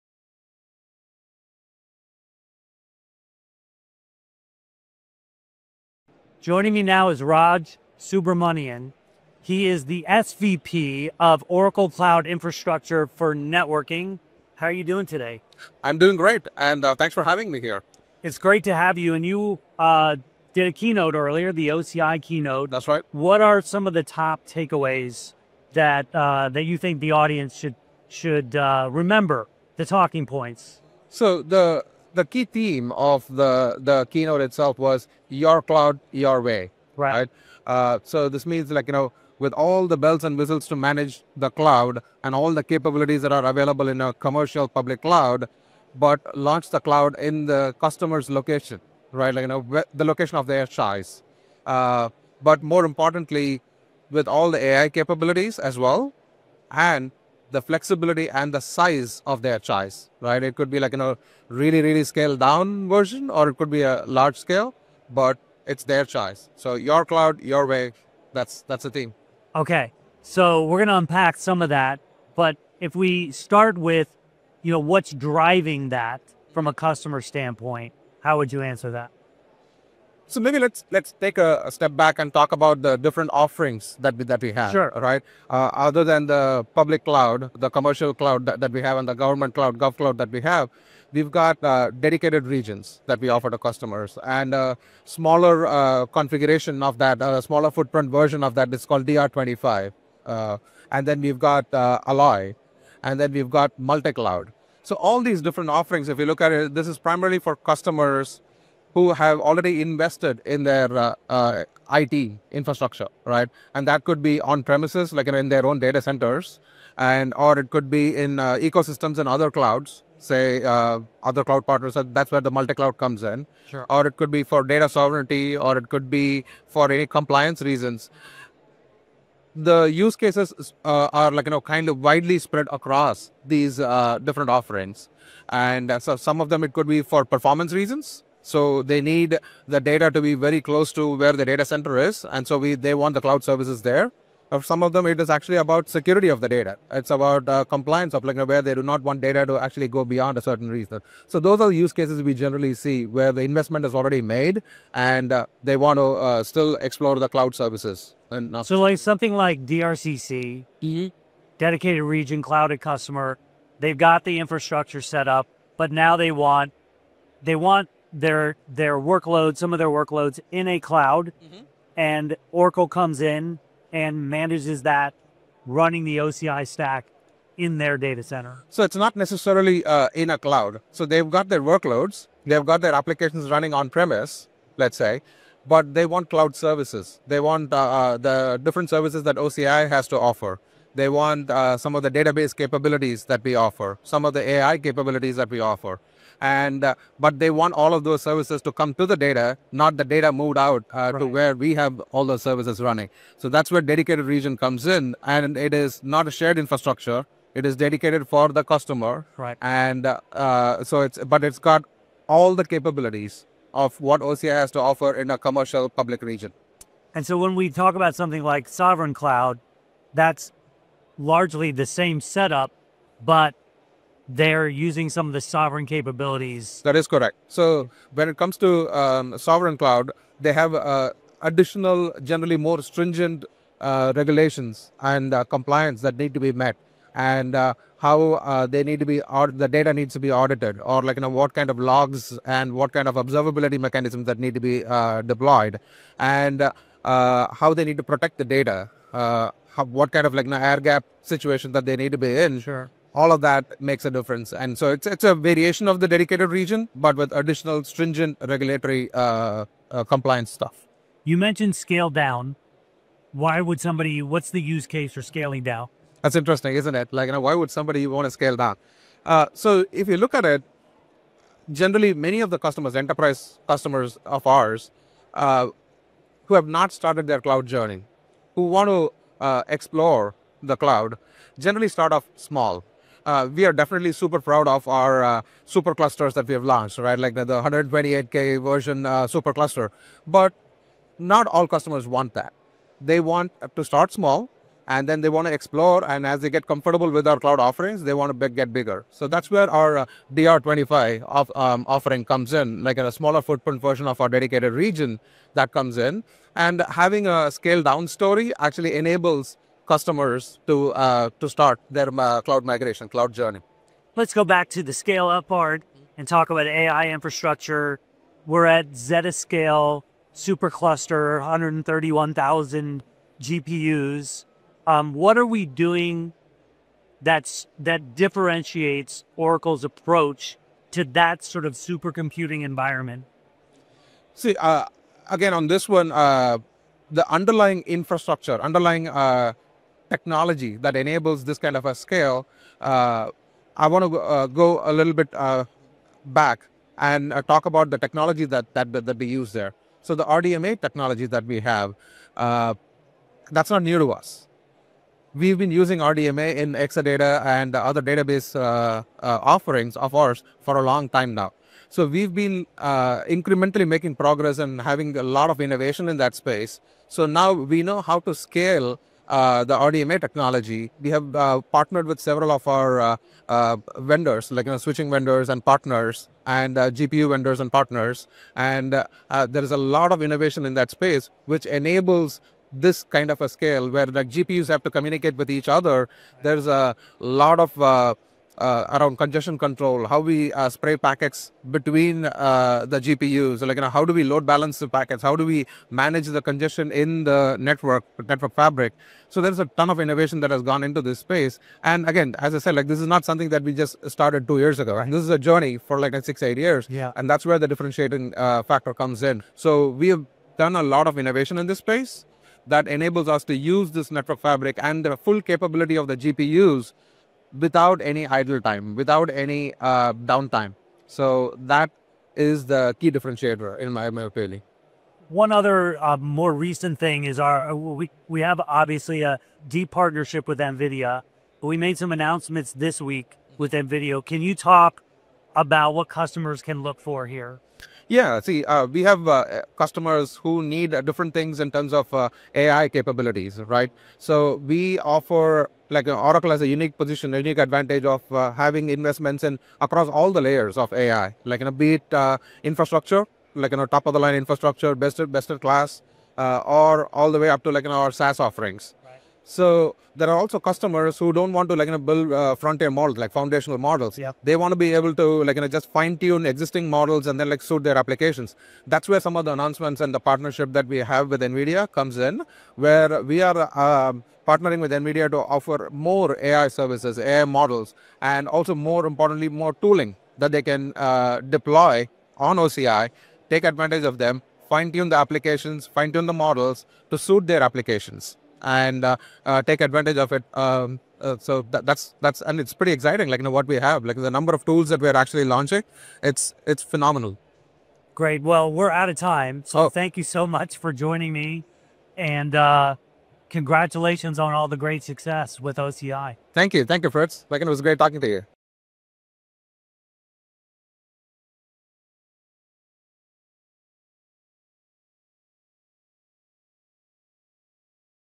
OCI Provides AI Infrastructure, Flexibility, and Security: Oracle TV CloudWorld Tour London 2025
Missed out on CloudWorld Tour London? Here are more upcoming events

Rajagopal Subramaniyan, SVP of OCI Networking, joins Oracle TV at CloudWorld Tour London to explain how we’re helping customers scale with Oracle's distributed cloud. We'll also cover how EMEA customers are benefitting from dedicated regions, Oracle's multicloud partnerships, and Oracle's AI infrastructure. Finally, we'll share how customers are leveraging our partnership with NVIDIA to drive greater innovations in AI.